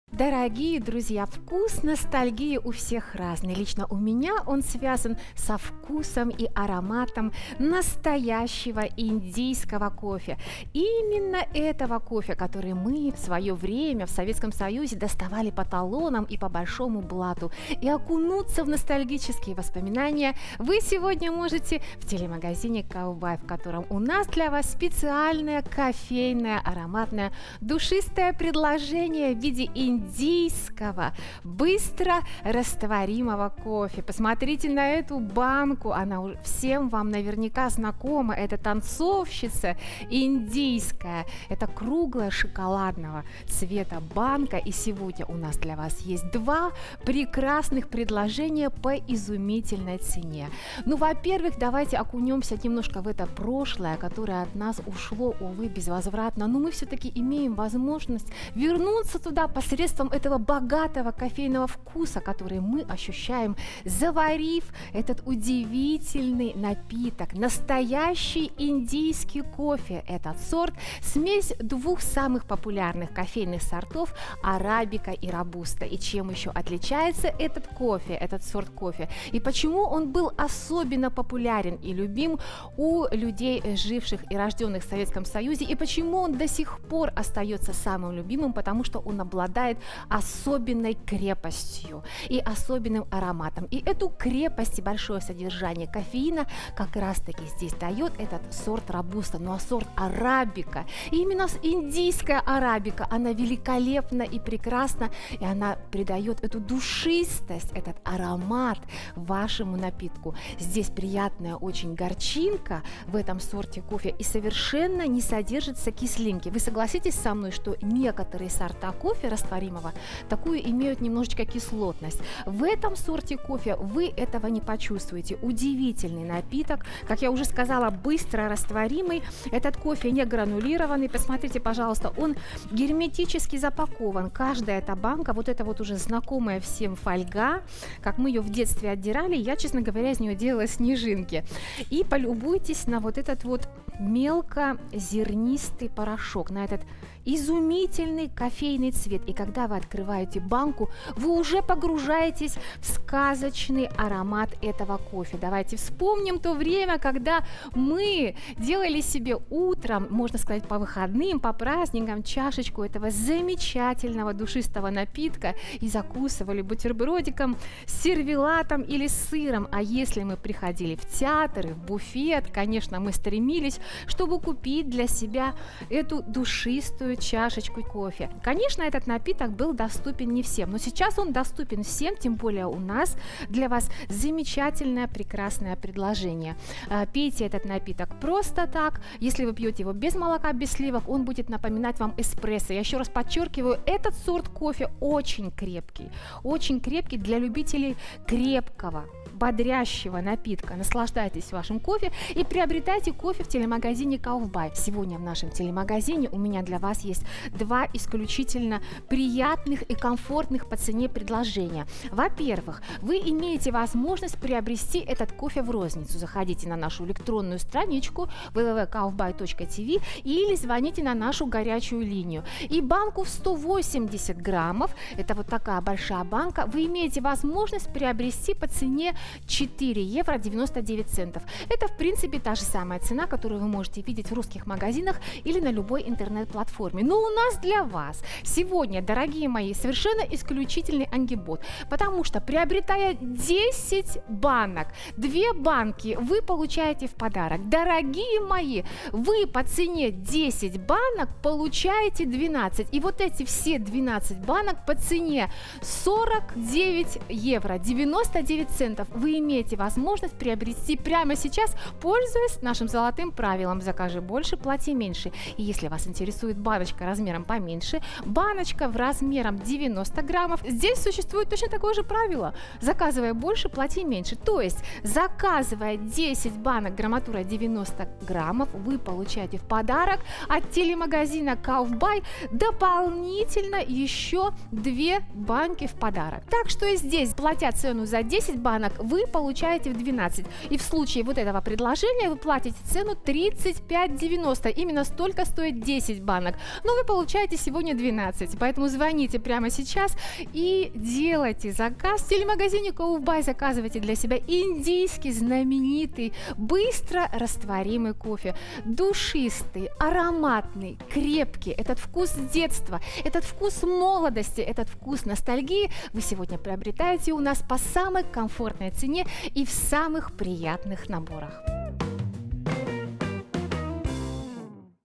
Индийский растворимый кофе
Предлагаемый нами индийский растворимый кофе обладает неповторимым вкусом и ароматом, который достигается благодаря уникальному климату Индии и природной среде кофейных плантаций.

Прежде всего, этот кофе обладает самым узнаваемым и богатым вкусом кофейных зерен арабики и робусты! Это лучшие сорта для характерного индийского кофе, который известен еще с давних времен.

В свое время этот кофе был настоящим хитом экспорта и очень популярен в странах бывшего Восточного блока - настоящий ностальгический продукт!

Индийский растворимый кофе обладает необычным, но очень приятным вкусом. С чашечкой этого кофе ваш день будет полон энергии и хорошего настроения!

Продукт поставляется в классической металлической банке

Способ применения: в зависимости от вашего вкуса, залейте кипятком 2-3 кофейные ложки и сразу же размешайте - готово! Добавьте молоко и сахар по вкусу.

Импортер в ЕС: Trade Group LTD, 401c Brivibas, Riga, LV-1024 Latvia
Произведено и упаковано в Индии компанией SLN COFFEE (P) LIMITED,
Кудлур, 571234 Кушалнагар

В 100 г содержится: Энергетическая ценность 1423 кДж / 340 ккал
Жир 0 г
в том числе насыщенные жирные кислоты 0 г
Углеводы 65 г
из них сахара 0 г
Белки 20 г

Ингредиенты: Индийский растворимый кофе, изготовленный из сортов Арабика и Робуста

Срок годности: 24 месяца

Объем поставки:
10х индийский растворимый кофе á 90г
2х бесплатный индийский растворимый кофе á 90г